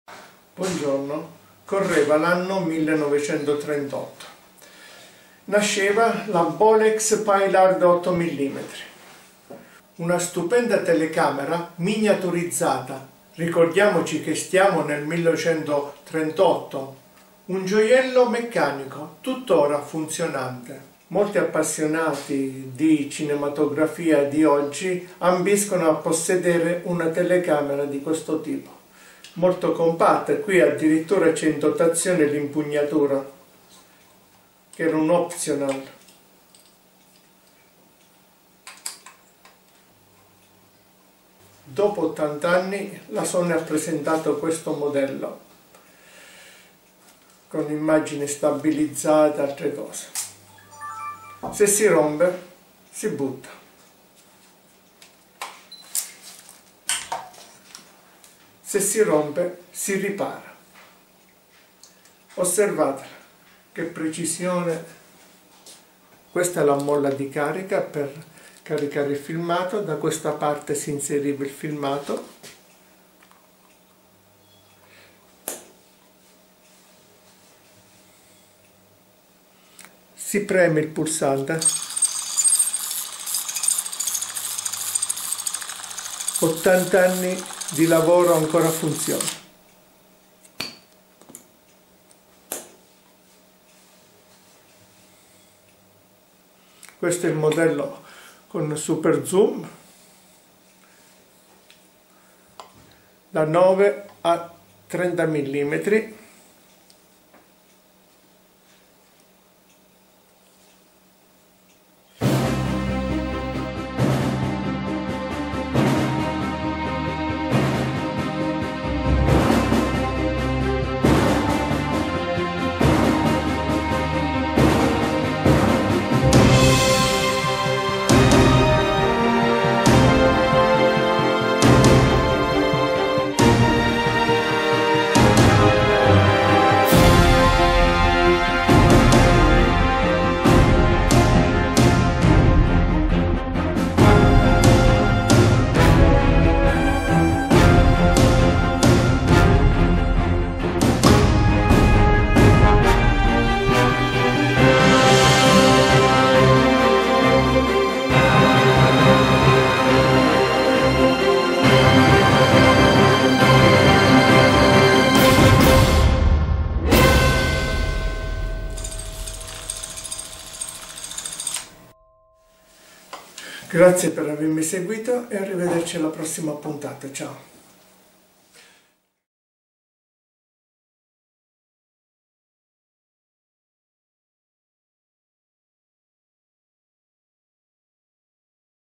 Bolex Paillard 80 anni e non li dimostra. Apparecchi fatti per durare nel tempo
Riparazioni e manutenzioni strumento ottici meccanici.